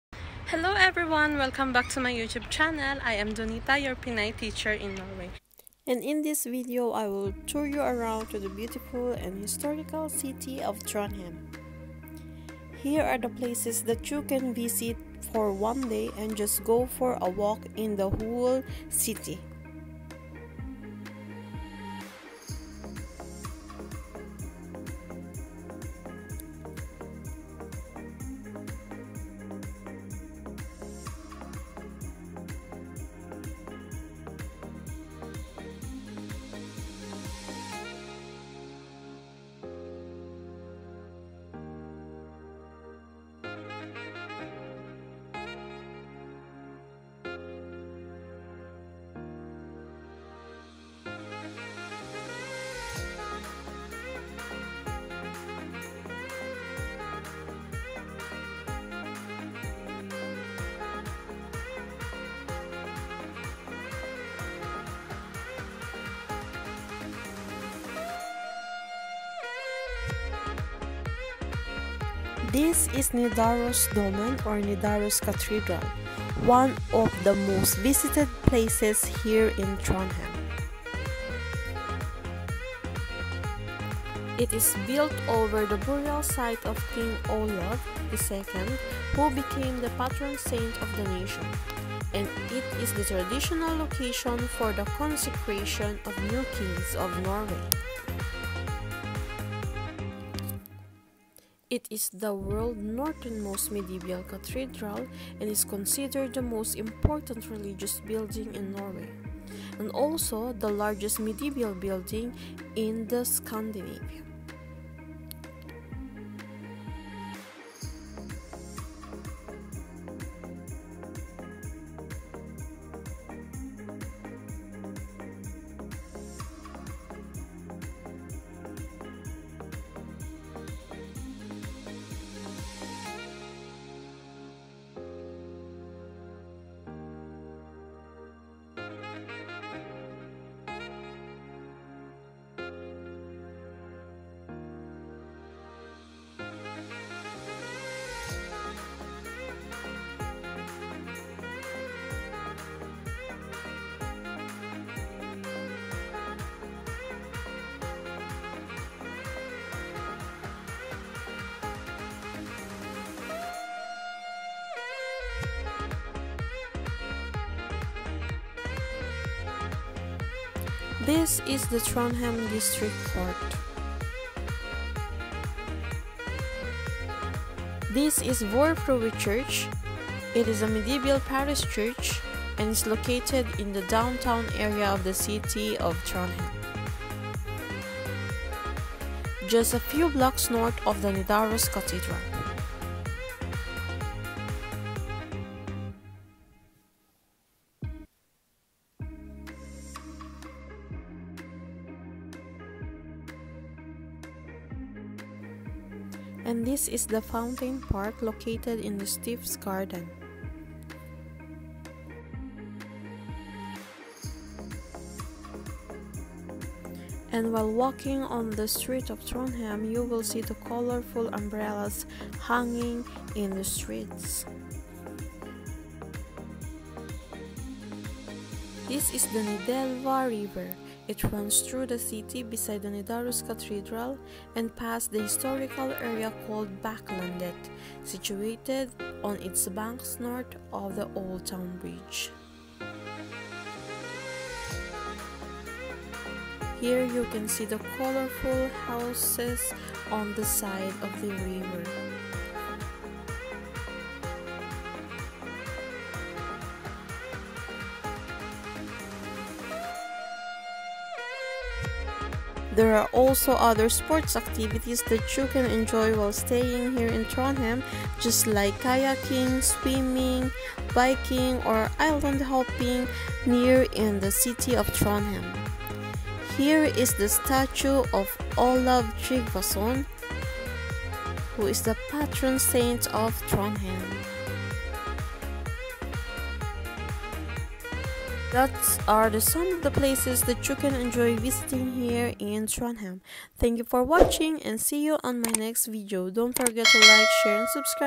VIRTUAL TOUR IN TRONDHEIM NORWAY
Virtual city tour in Trondheim Norway with Pinay teacher in Norway!
Did you know that you can explore Trondheim City by walking in just one day. Here is my experience.
Click the Hd settings for a High definition quality of this video!